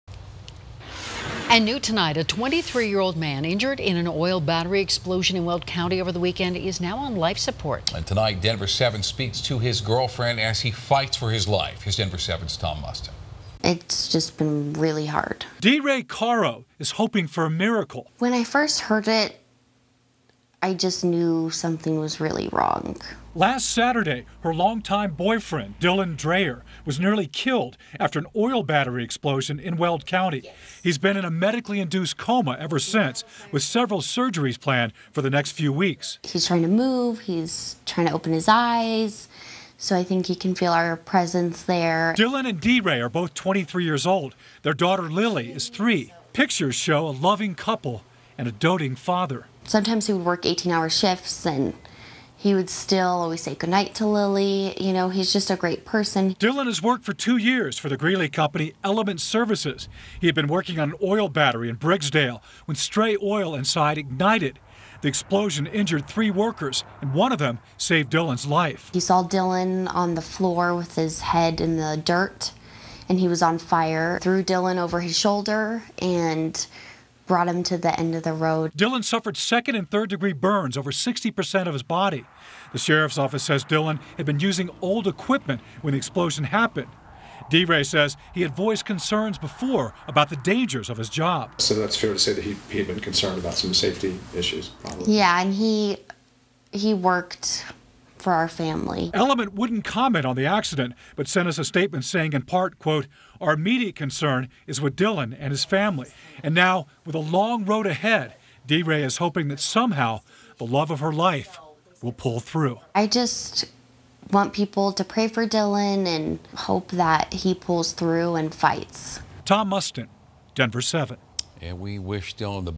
Loveland father fighting for his life after oil tank explosion
An oil tank worker is on life support after an oil battery explosion in Weld County. It happened Saturday in Briggsdale.